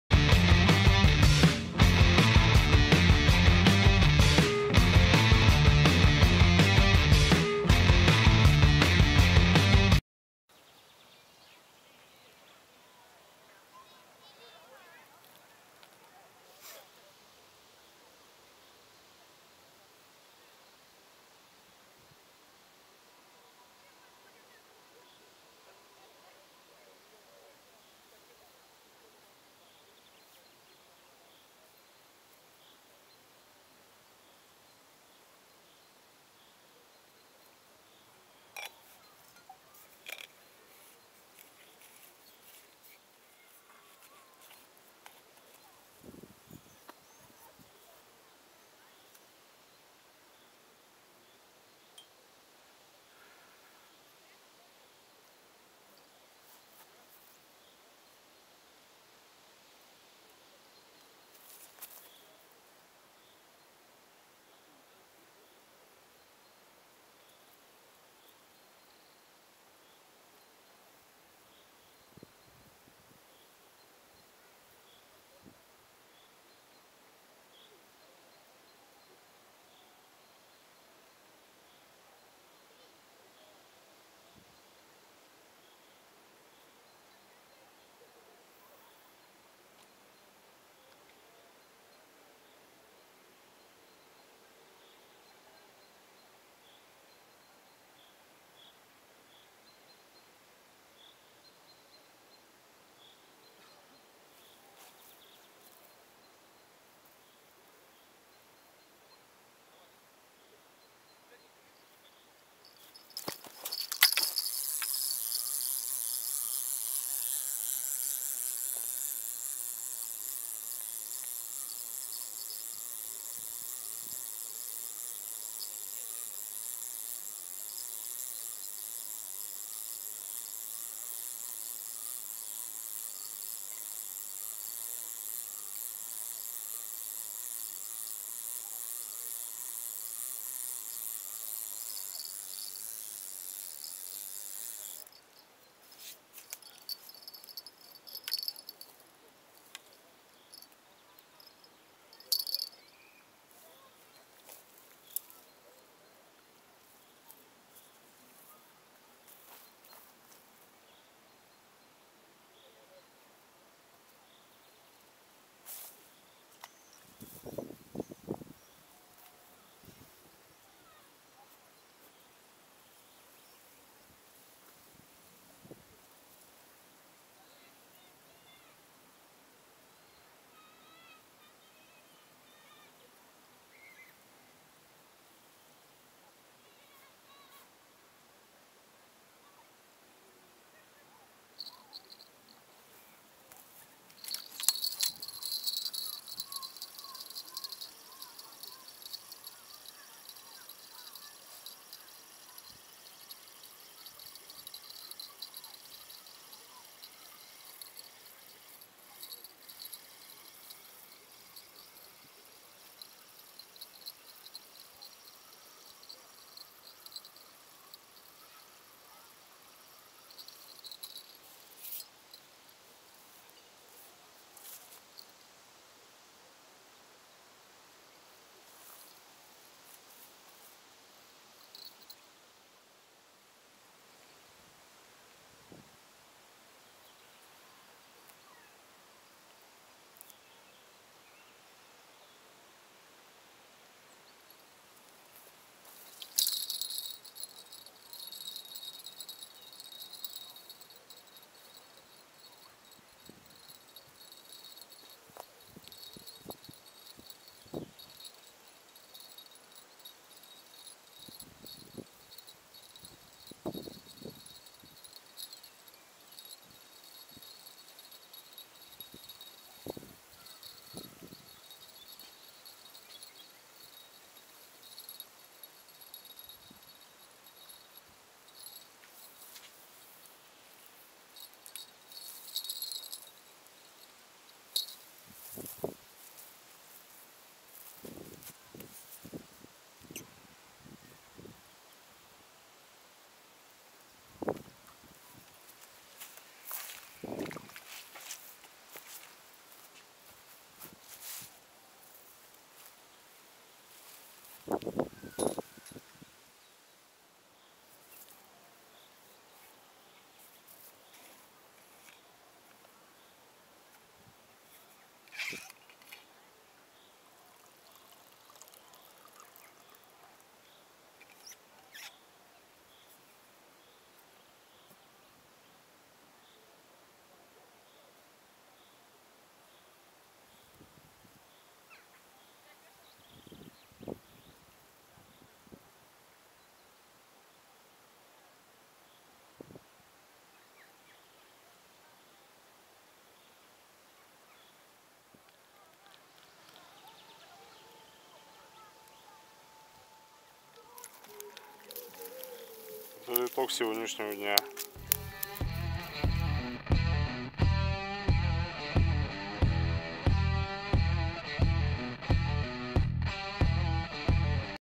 Подмосковное серебро или ловля подлещика на донки
Ловля подлещика на донки...Прикормка самодельная.Насадка опарыш,мотыль.